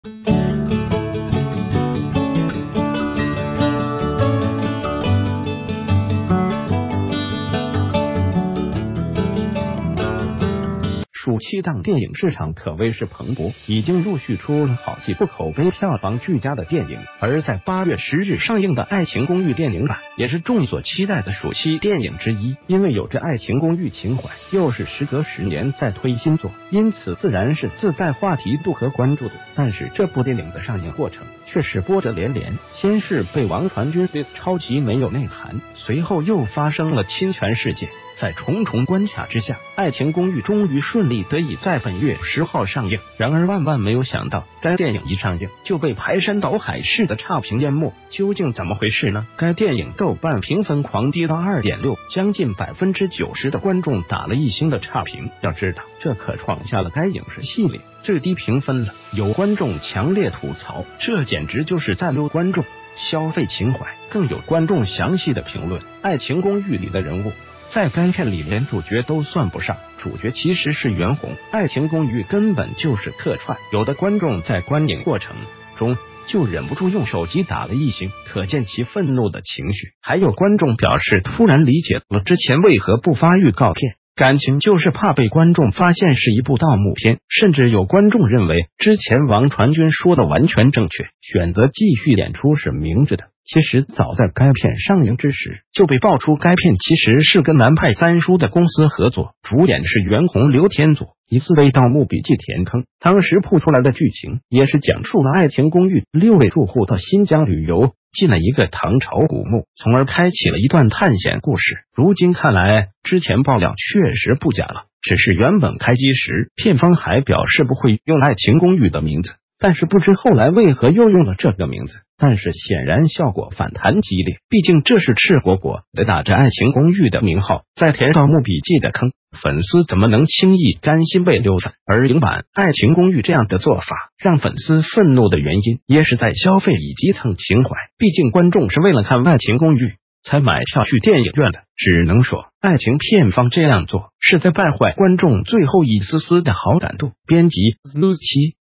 《愛情公寓》大撲街！觀眾暴怒打一星差評，消費情懷可有底線？
暑期檔電影市場可謂是蓬勃，已經陸續出了好幾部口碑票房俱佳的電影。而在8月10日上映的《愛情公寓》電影版也是眾所期待的暑期電影之一，因為有著愛情公寓情懷，又是時隔十年再推新作，因此自然是自帶話題度和關注度。但是這部電影的上映過程，卻是波折連連，先是被王傳君diss抄襲沒有內涵，隨后又發生了侵權事件。
    在重重關卡之下，《愛情公寓》終于順利得以在本月10號上映，然而萬萬沒有想到，該電影一上映，就被排山倒海式的差評淹沒。究竟怎么回事呢？
    該電影豆瓣評分狂跌到2.6，將近90% 的觀眾打了一星的差評。要知道，這可創下了該影視系列，最低評分了。有觀眾強烈吐槽，這簡直就是在溜觀眾、消費情懷。
    更有觀眾詳細的評論，愛情公寓里的人物在該片里連主角都算不上，主角其實是袁弘，愛情公寓根本就是客串！有的觀眾在觀影過程中就忍不住用手機打了一星，可見其憤怒的情緒。
    還有觀眾表示突然理解了之前為何不發預告片，敢情就是怕被觀眾發現是一部盜墓片！甚至有觀眾認為，之前王傳君說的完全正確，選擇繼續演出是明智的。
    其實早在該片上映之時，就被爆出該片其實是跟南派三叔的公司合作，主演是袁弘劉天佐，疑似為盜墓筆記填坑。
    當時曝出來的劇情，也是講述了愛情公寓六位住戶到新疆旅游，進了一個唐朝古墓，從而開啟了一段探險故事。
    如今看來，之前爆料確實不假了。只是原本開機時，片方還表示不會用愛情公寓的名字，但是不知后來為何又用了這個名字，但是顯然效果反彈激烈，畢竟這是赤果果的打著愛情公寓的名號，在填盜墓筆記的坑，粉絲怎么能輕易甘心被溜粉？
    而影版《愛情公寓》這樣的做法，讓粉絲憤怒的原因，也是在消費以及蹭情懷。畢竟觀眾是為了看《愛情公寓》才買票去電影院的。只能說，《愛情》片方這樣做，是在敗壞觀眾最后一絲絲的好感度。
    編輯：xnnews7